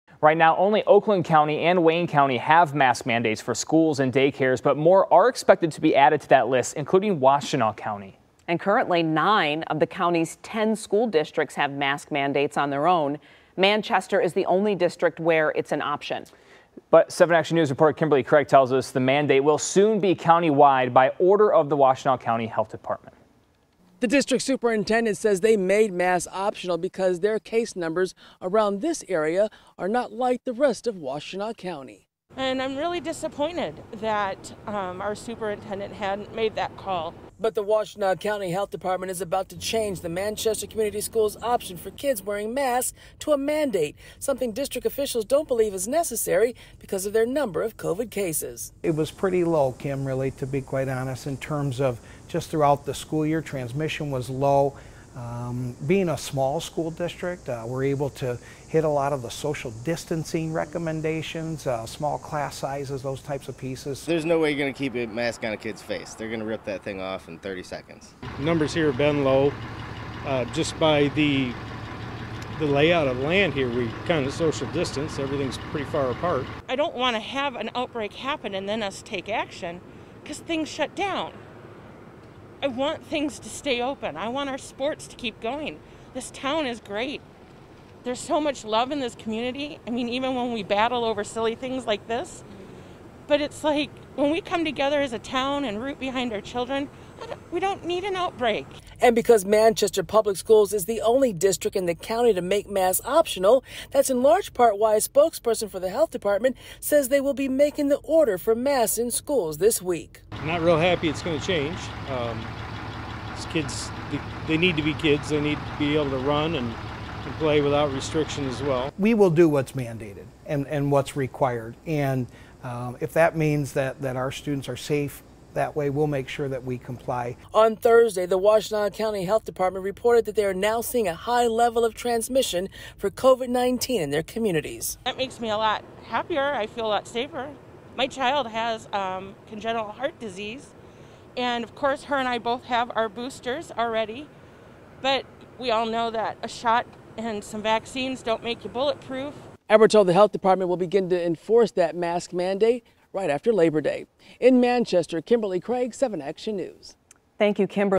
School district's optional mask policy prompts health department to mandate it
The Manchester school district's optional mask policy prompts the Washtenaw County Health Department to mandate them.